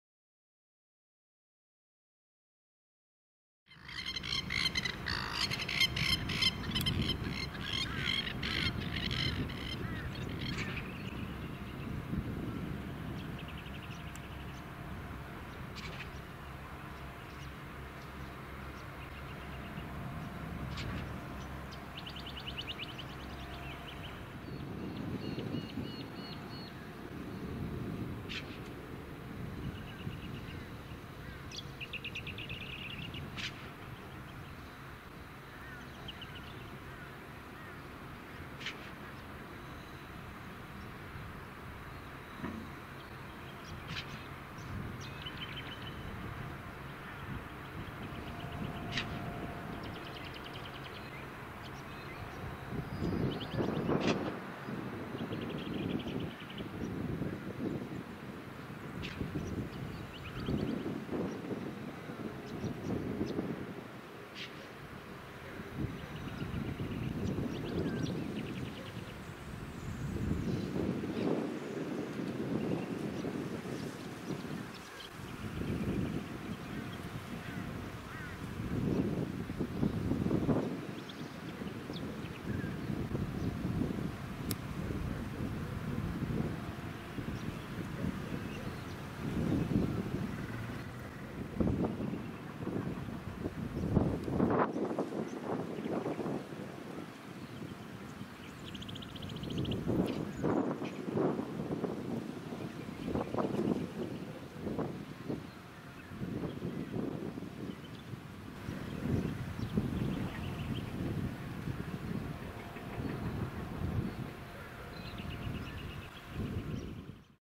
Masked Lapwing 7
Masked Lapwing, more commonly called a Plover. Scientific name: Vanellus miles. Filmed, in bushland that is regenerating after a recent bushfire, near a suburban development in the Ipswich region, Queensland, in September 2018. This video includes the sound of birdsong, the wind and suburban noises